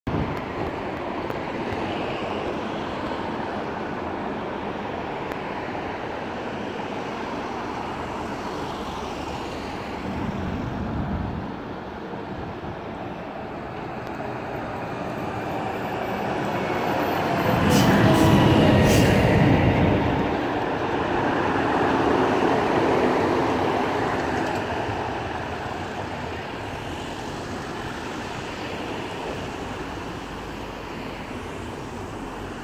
Tampereen ensimmäinen ratikka Lahdessa
Tampereen ensimmäinen raitiovaunu matkalla Kajaanista Tampereelle lauantaina 23. toukokuuta 2020.